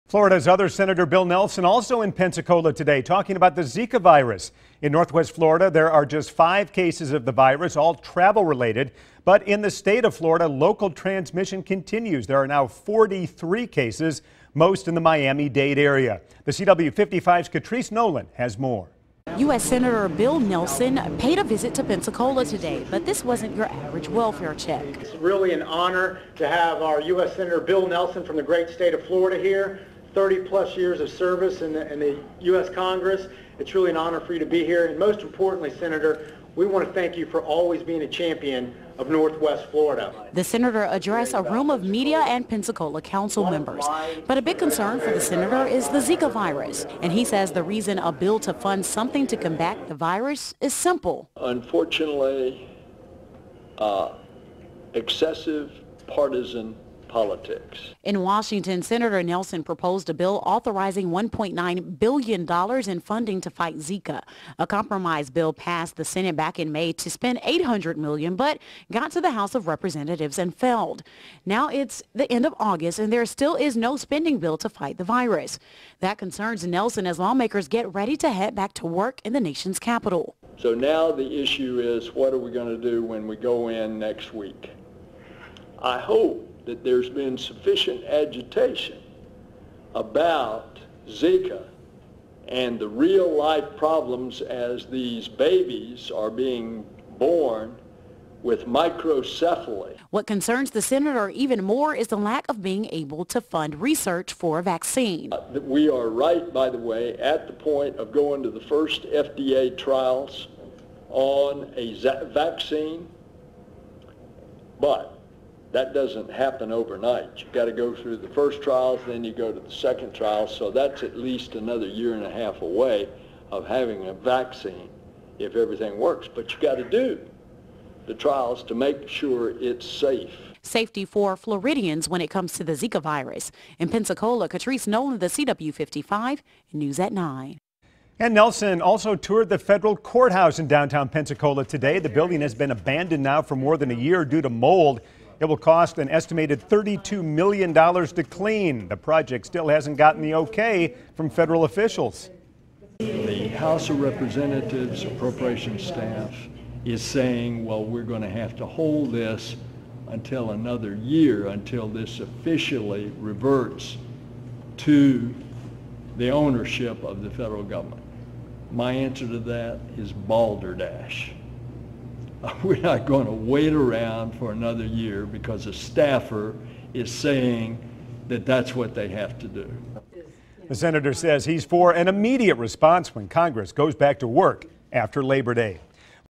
Bill Nelson Discusses Zika, Tours Courthouse
CW55's Katrice Nolan reports.